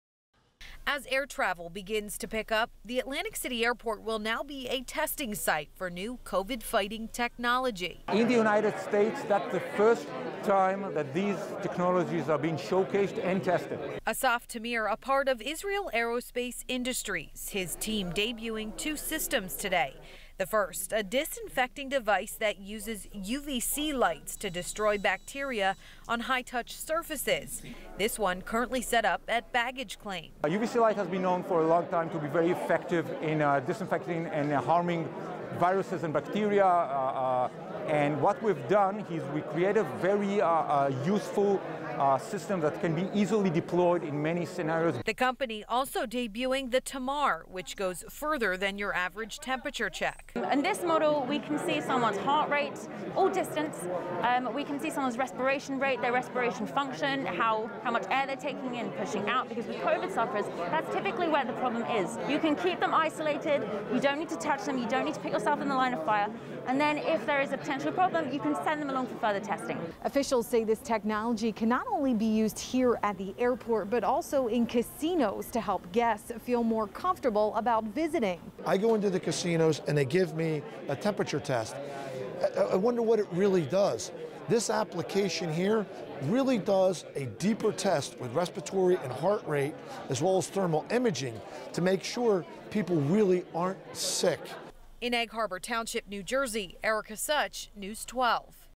Atlantic City Int’l Airport debuts new technology to protect travelers against COVID-19
The Atlantic City International Airport is now using new technology to help travelers feel safer during the pandemic.